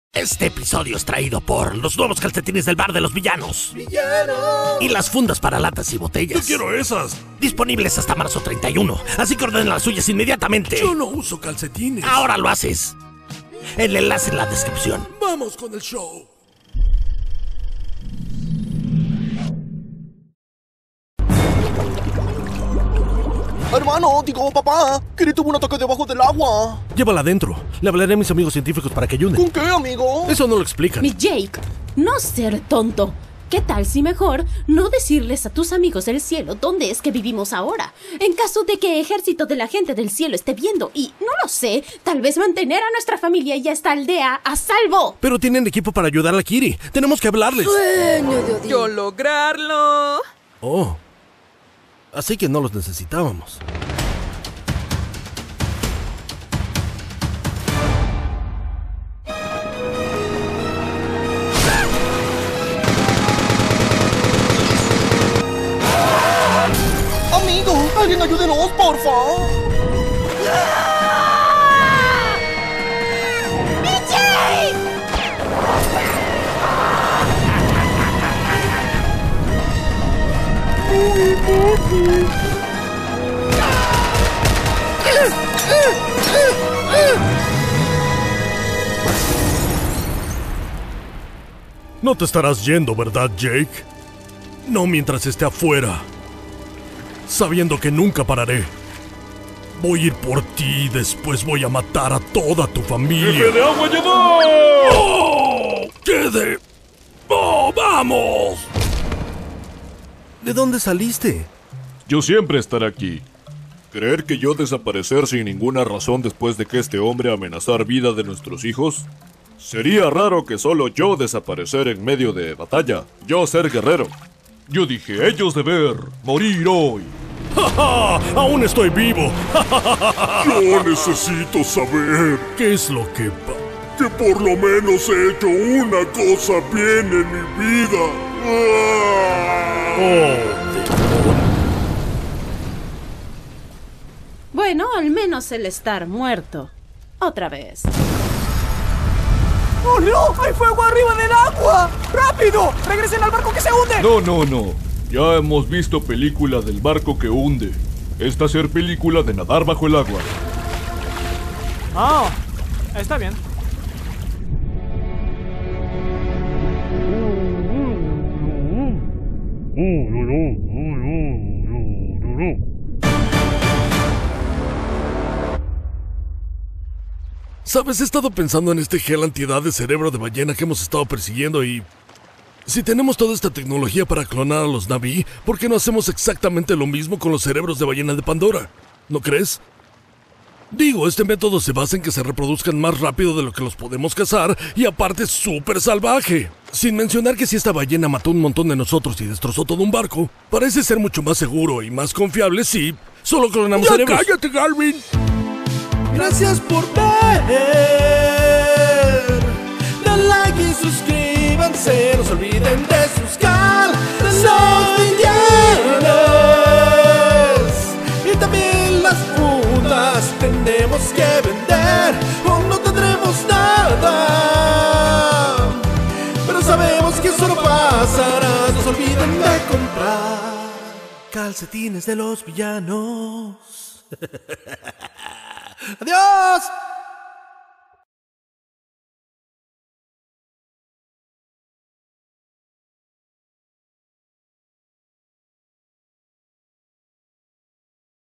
Como Avatar El Camino del Agua Debería Haber Terminado
Ya regresamos y que mejor que con Como Avatar El Camino del Agua Debería Haber Terminado, sabemos que era uno de los que más esperaban, y no crean que se nos olvida, ya estamos grabando Black Panther 2, así que espérenlo pronto, por mientras ojalá disfruten este video tanto como nosotros al hacer el doblaje, Saludos a todos =D

Obtengan sus Calcetines del Bar de los Villanos a partir del 31 de Marzo, pero pueden ordenarlos desde ahora!

Tiktok Ingles @HISHEofficial

Voces
Luis Horacio González
Palpatine, Zod, Voldemort, Jake, Quaritch, Ballena Pandora, Dr. Garvin, Cancion Calcetines.

Erika Martinez
Kiri

Voces Invitadas

Pilar Soto
Neytiri.

Chema García Alexandre
Hijo de Jake y Neytiri.

Charlie Leng
Jefe del Agua.

Joaquín Augusto
Spider.

Videos Anteriores